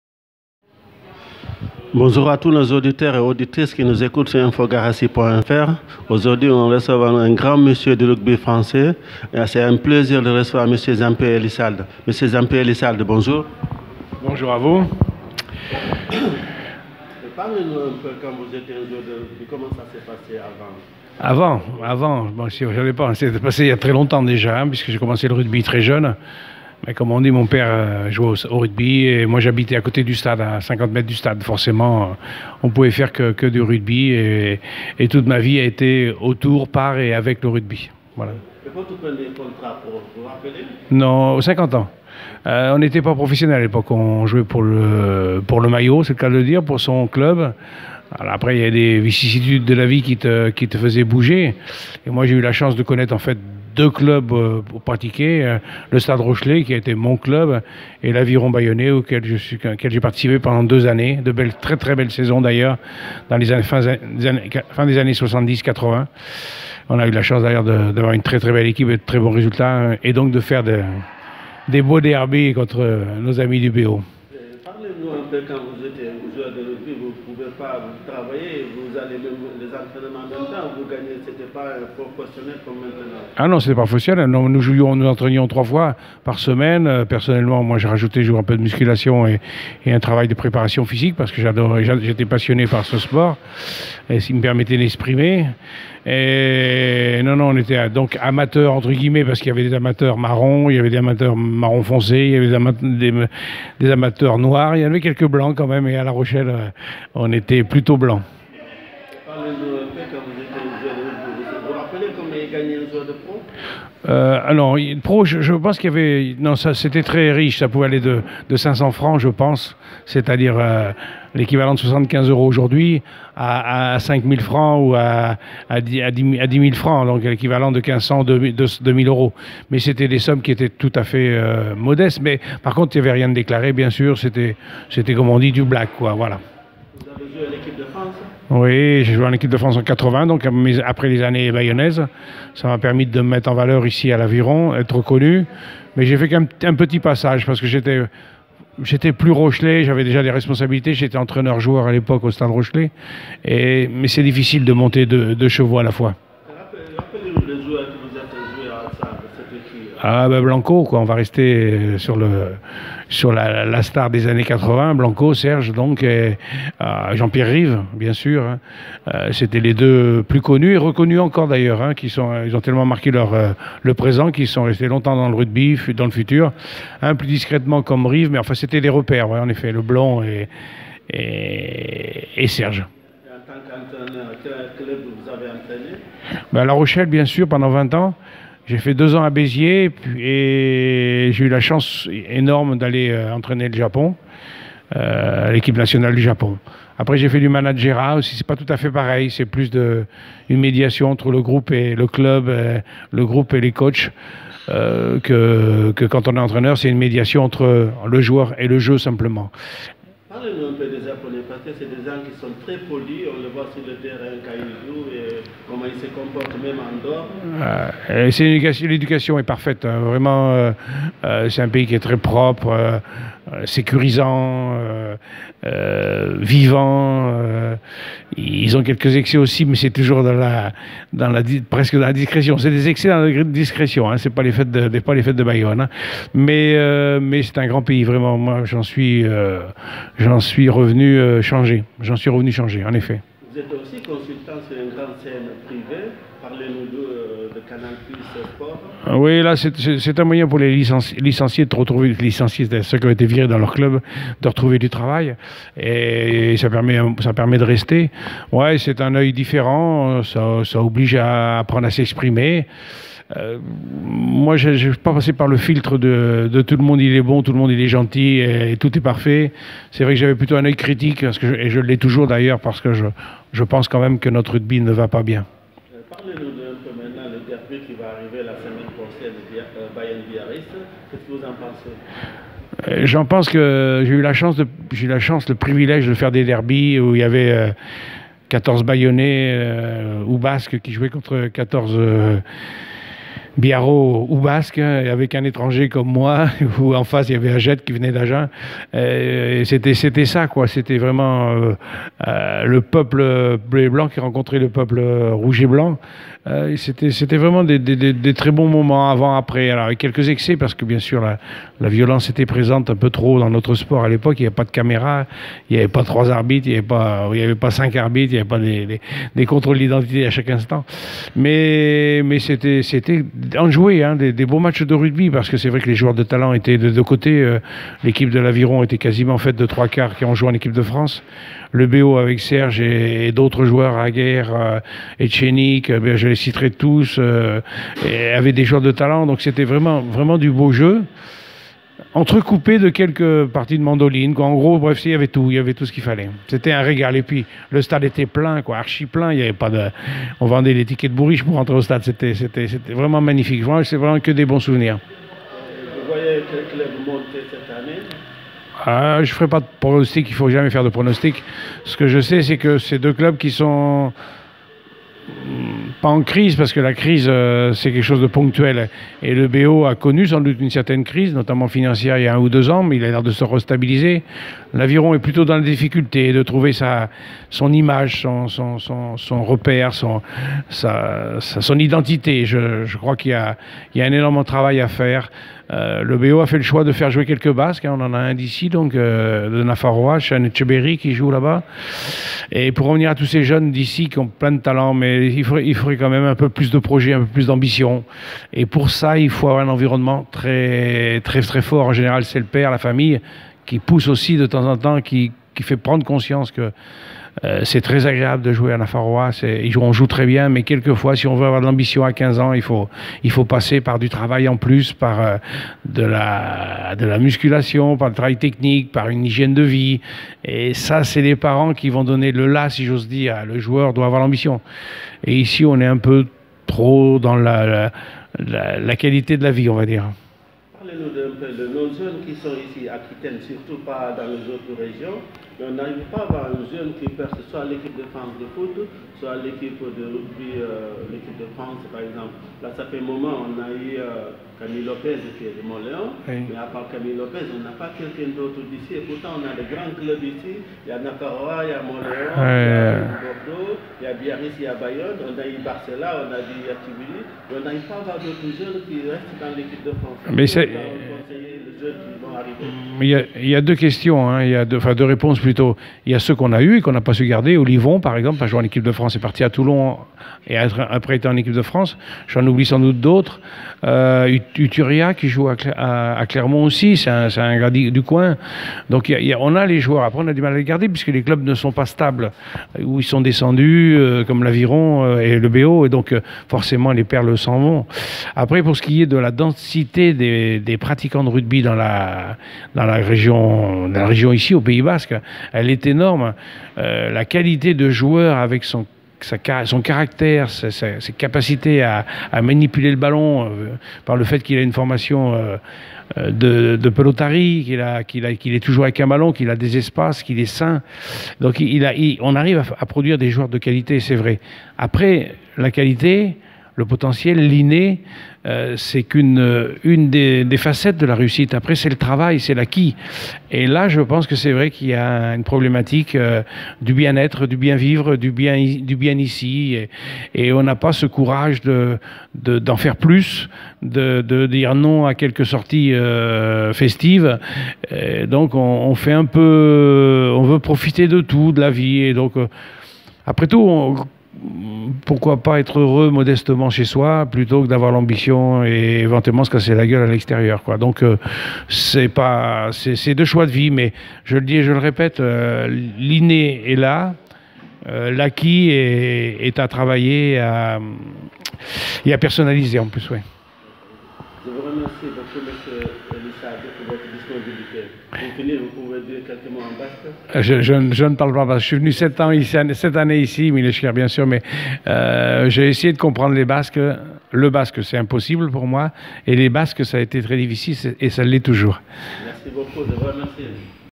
Interview Jean-Pierre ELISSALDE
Ancien joueur de l équipe de France et ancien entraîneur Bayonne et la Rochelle